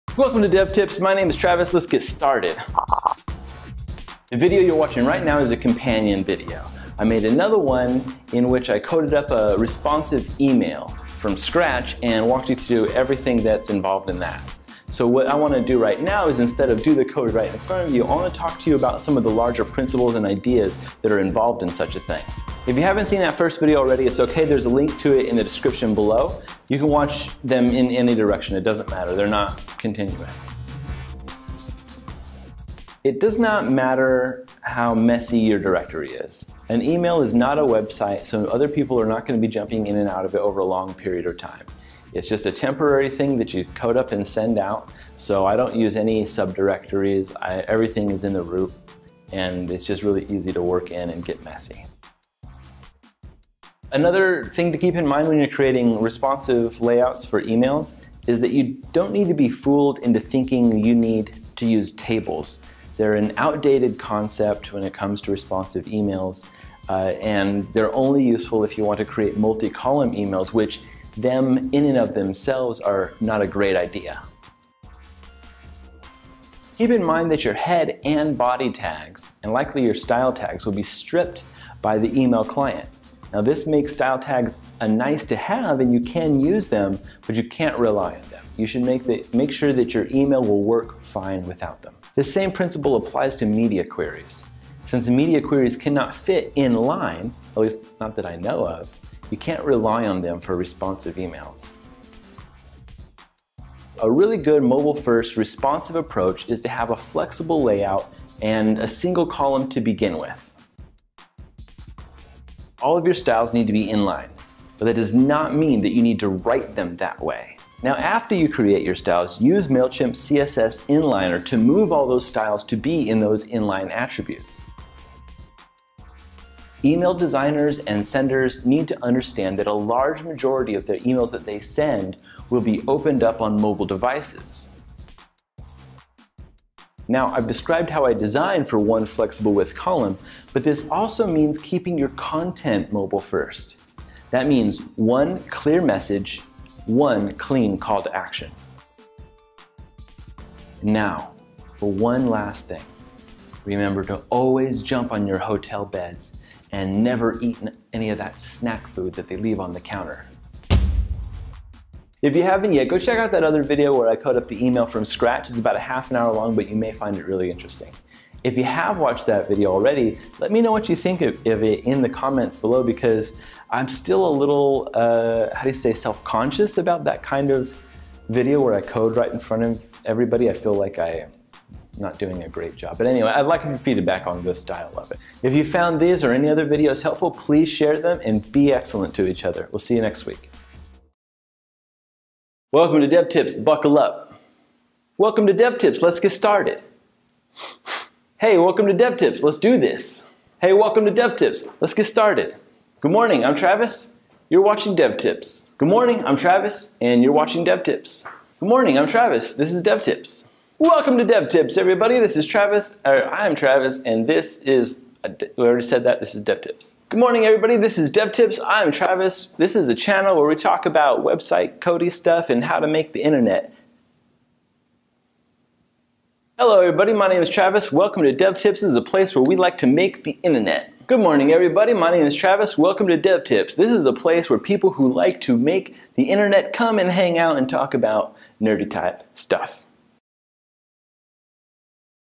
How to Code a Responsive HTML Email — a few thoughts
This is a companion to this video, in which we code up and style a responsive HTML Email from scratch

This video covers a few main points:

# Coding structure

- It doesn't matter, don't worry about directories and junk, you want to get this done fast and dirty.

- Don't be fooled into thinking that you need to use tables. That's an outdated practice and is only useful for creating multi-column layouts, which are themselves, not a great idea...

- Keep in mind that your head and body tags will likely be stripped by the email client. This makes style tags a nice-to-have, but you can't rely on them.

- Which makes @media queries unreliable also.

- Which means that a good responsive email is naturally flexible, and laid out in a single column to begin with.

- All of your styles need to be inline. But that does not mean that you need to write them that way.

- Use mailchimp's css inliner to move the css inline after you are done creating it using what ever method you prefer. I use SASS. Like a boss.

# Mobile first messaging

Email designers and senders really need to understand and accept that a large majority of their emails will be opened up on a mobile device. I've described how I design for one flexible width column. But this also means keeping your content mobile-first.
- One clear message.
- One clear call to action.